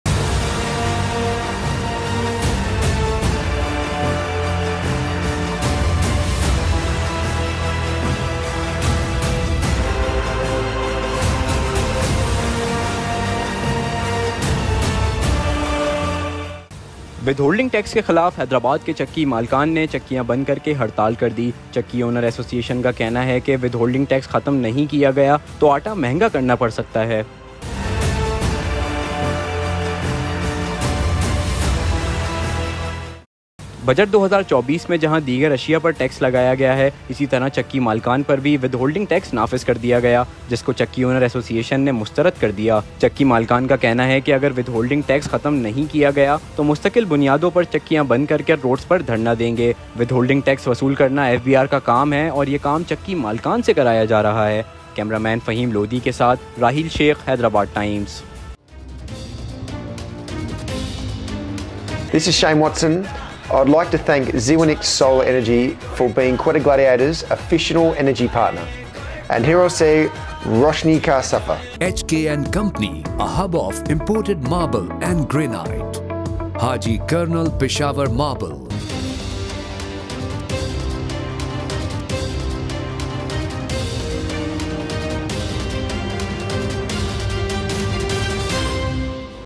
Flour mills closed in Hyderabad Fear of increase in the price of flour per kg
حیدرآباد میں آٹا چکیاں بند
آٹے کی فی کلو قیمت میں اضافے کا خدشہ